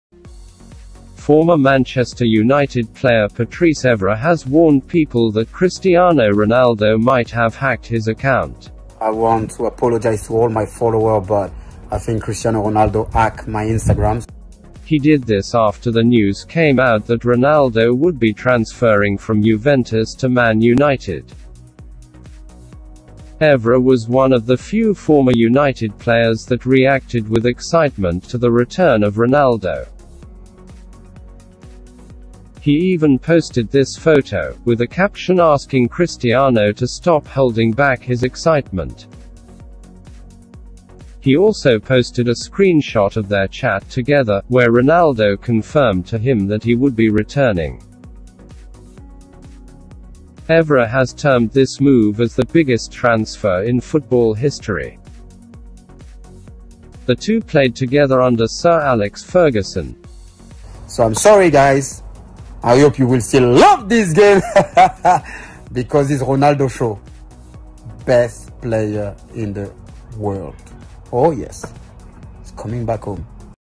RONALDO HACKED MY INSTAGRAM - Patrice Evra
Former Manchester United player Patrice Evra has warned people that Christiano Ronaldo might have hacked his Instagram account.

He did this after the News came out that Ronaldo would be transferring from Juventus to Manchester Utd squad of Ole Gunnar Solskjær.

Evra was one of the few former Man Utd players that reacted with excitement to Ronaldo's return to Carrington training ground.

Evra has termed this move as the 'Biggest transfer in football history'.

The two played together under Sir Alex Ferguson.

Ronaldo debut and presentation in Man United vs Newcastle at Old Trafford.